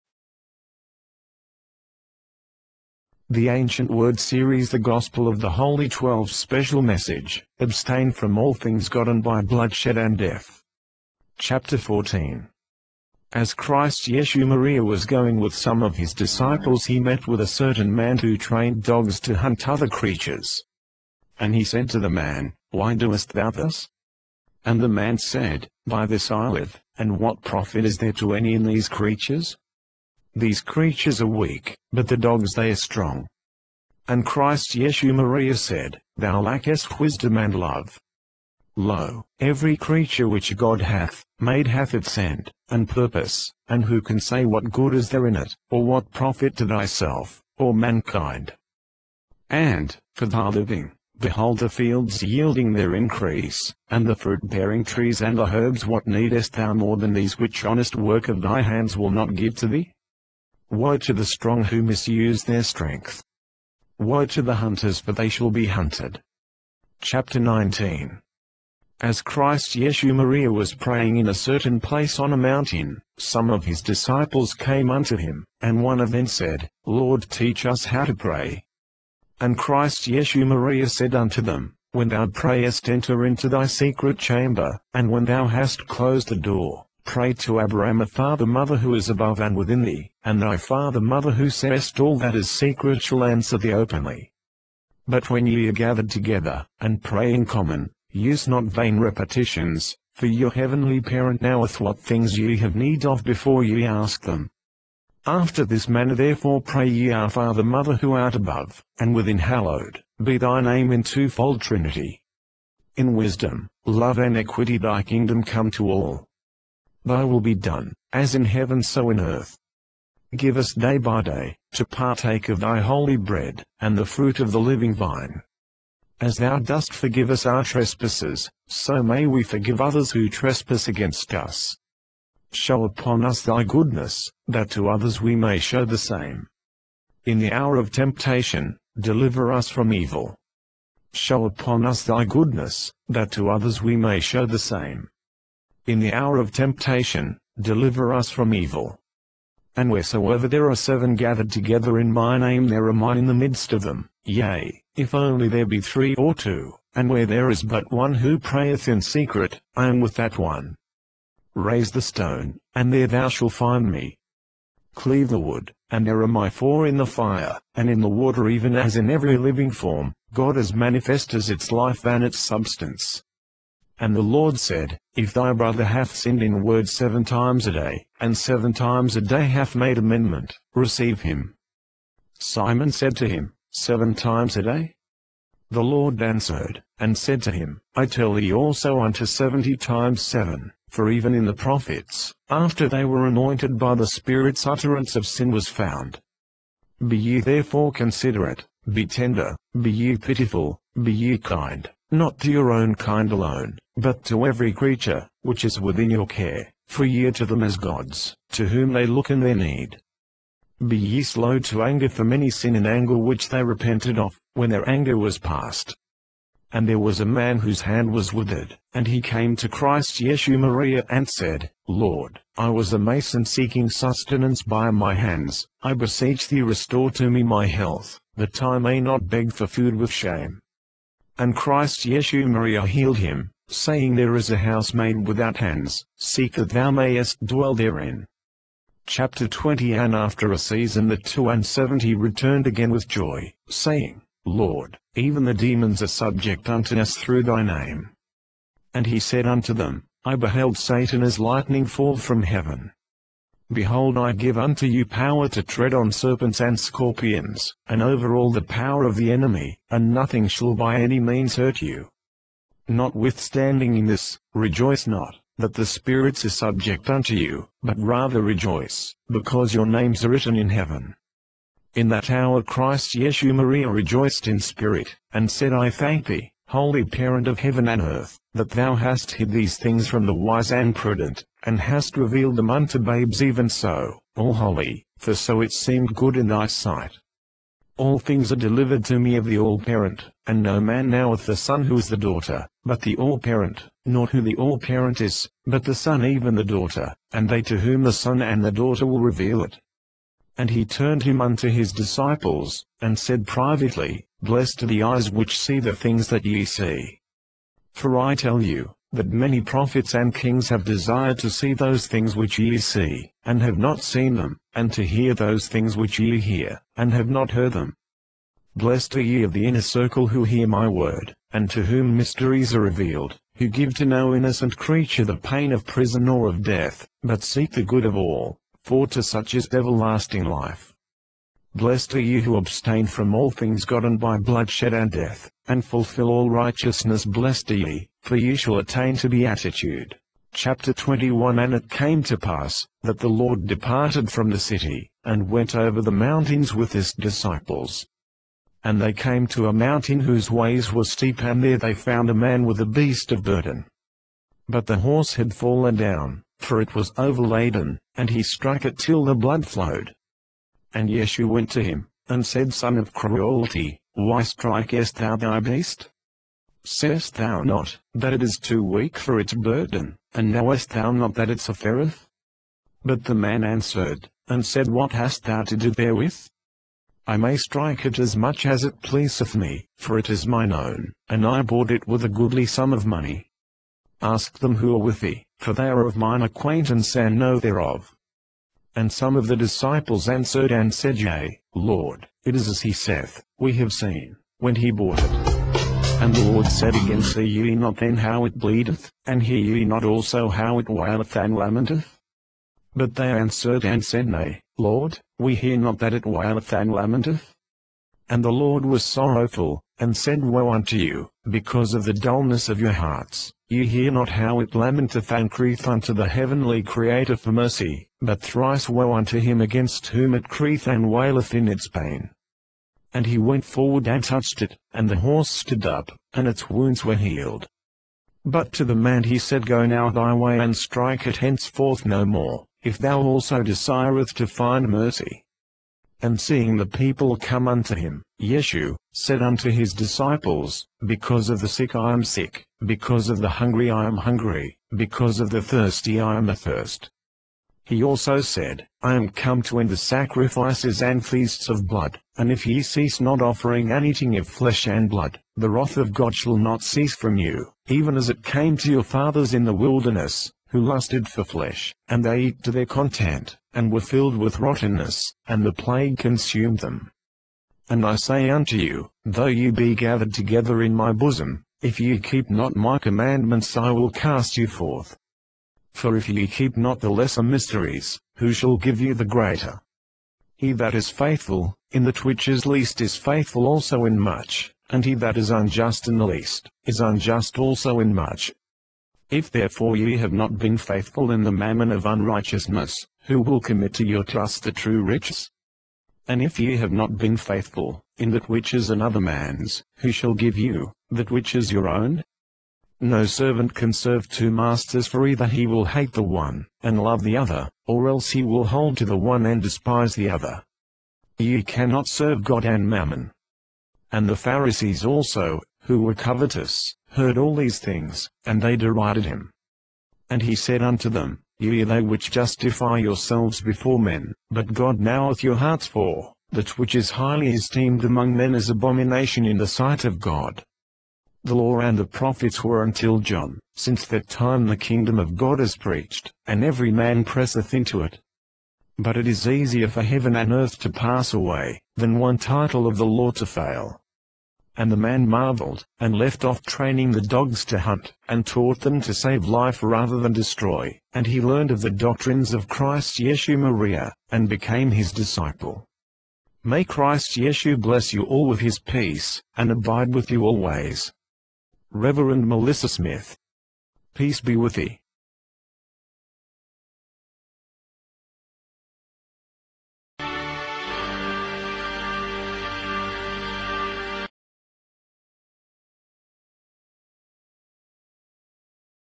Meat - Your Destiny with Ill Health and Disease
Excerpt from The Gospel of the Holy Twelve:
"And some of his disciples came and told him of a certain Egyptian, a son of Belial, who taught that it was lawful to torment animals, if their sufferings brought any profit to men. And Christ Yeshu-Maria said unto them, "verily I say unto you, they who partake of benefits which are gotten by wronging one of God's creatures, cannot be righteous, nor can they touch holy things, or teach the mysteries of the kingdom, whose hands are stained with blood, or whose mouths are defiled with flesh. God giveth the grains and the fruits of the earth for food and for the righteous man truly there is no other lawful sustenance for the body." http:awstgh12.mypodcast.com